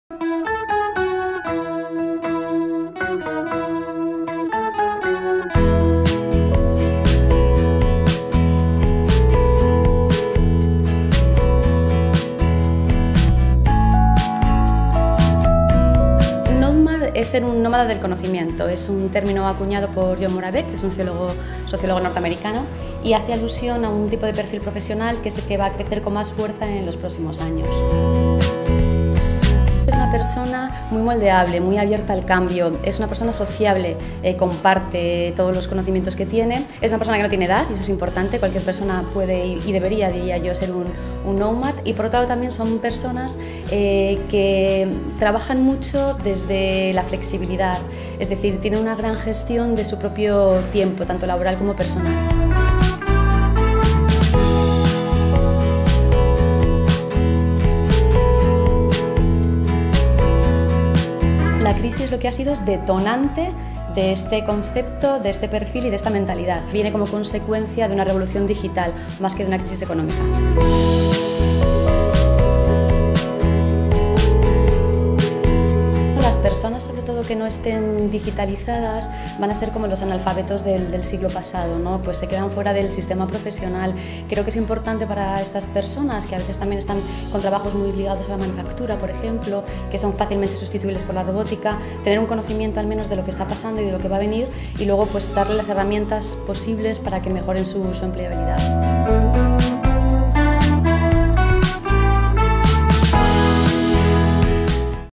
Así es el trabajador del futuro
Raquel Roca presenta en bez.es su libro "Knowmads. Los trabajadores del futuro", editado por Lid Empresarial. La obra aborda la aparición de los llamados knowmads, trabajadores del conocimiento que pueden trabajar desde cualquier lugar, en cualquier momento y con todo el mundo siendo valorados por contextualizar lo que saben para generar un valor nuevo.